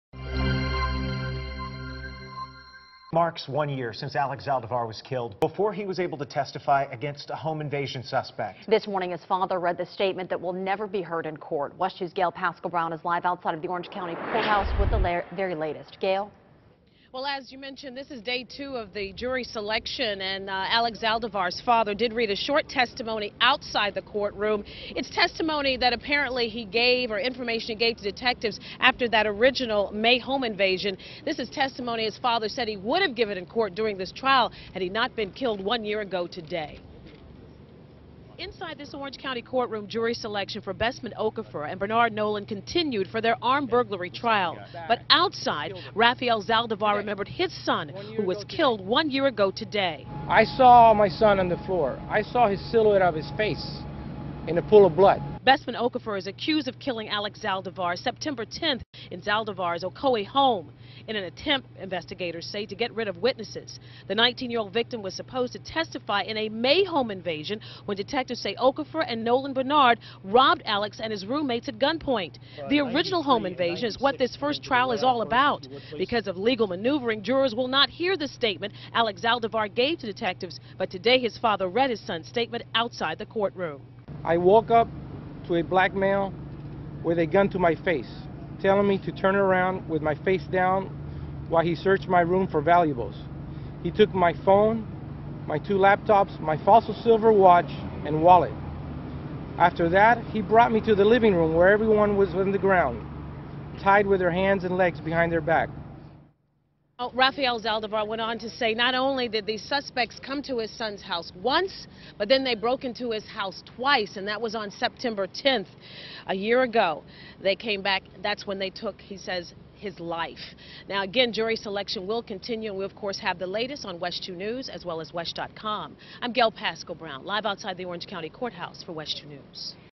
Zaldivar reads son's testimony outside courthouse
It's been one-year since Alex Zaldivar was shot and killed, all before he was able to testify against a home invasion suspect.